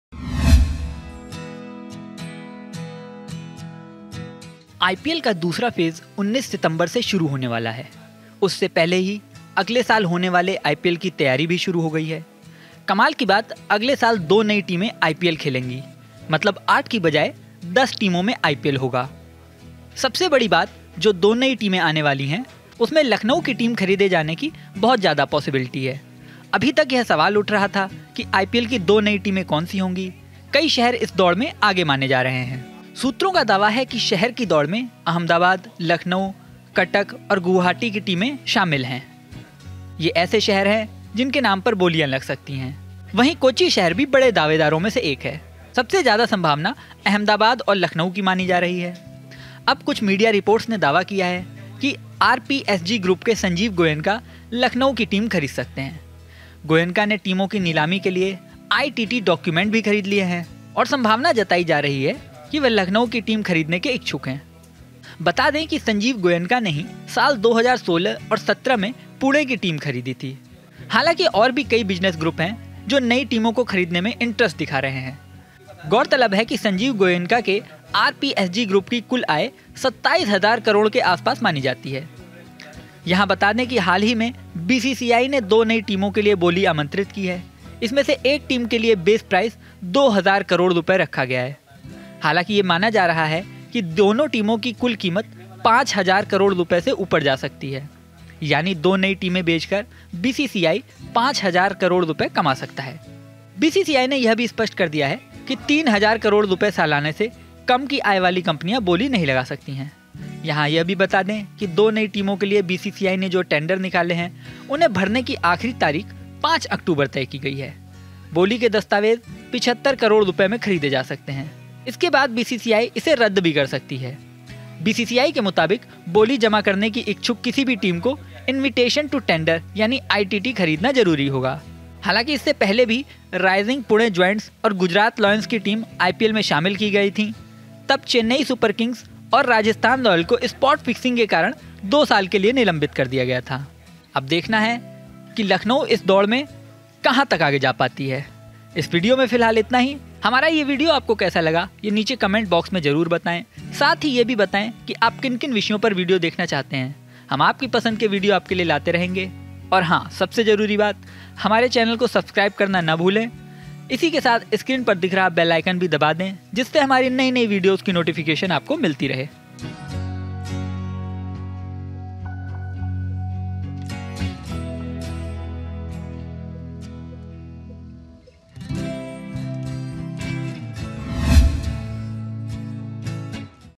IPL 2022: अगले आईपीएल में दिखेगी लखनऊ की टीम, ये करोड़पति है खरीदने को तैयार !
Lucknow team will be seen in the next IPL, ready to buy this millionaire!

IPL2022 में दो नई टीमें खेलती दिखाई देंगी. इनमें सबसे ज्यादा संभावना लखनऊ की टीम को लेकर जताई जा रही है. दावा किया गया है कि एक बड़ा बिजनेस ग्रुप लखनऊ की टीम खरीदने में इंट्रस्टेड है.

Lucknow Team, IPL, RPSG, Sanjiv Goenka, IPLLatest, IPLNew, News Team in IPL, lucknow team in IPL

आईपीएल (IPL 2021) का दूसरा फेज 19 सितंबर से शुरू होने वाला है. उससे पहले ही अगले साल होने वाले आईपीएल की तैयारी भी शुरू हो गई है. कमाल की बात अगले साल दो नई टीमें आईपीएल खेलेंगी. मतलब आठ की बजाय 10 टीमों में आईपीएल होगा. सबसे बड़ी बात जो दो नई टीमें आने वाली हैं उनमें लखनऊ की टीम खरीदे जाने की बहुत ज्यादा पॉसिबिलिटी है. अभी तक यह सवाल उठ रहा था कि आईपीएल की दो नई टीमें कौन सी होंगी. कई शहर इस में दौड़ में आगे माने जा रहे हैं.